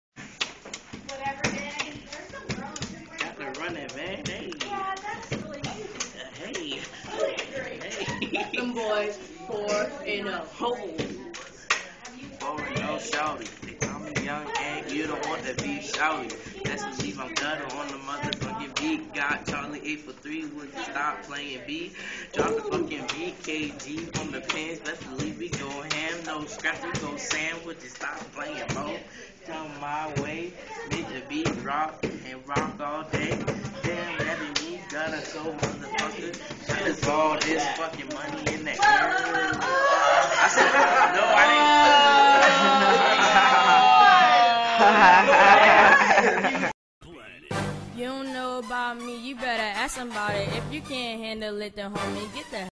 Bluffton high School Rapping in school
me and Big Ant freestyling in class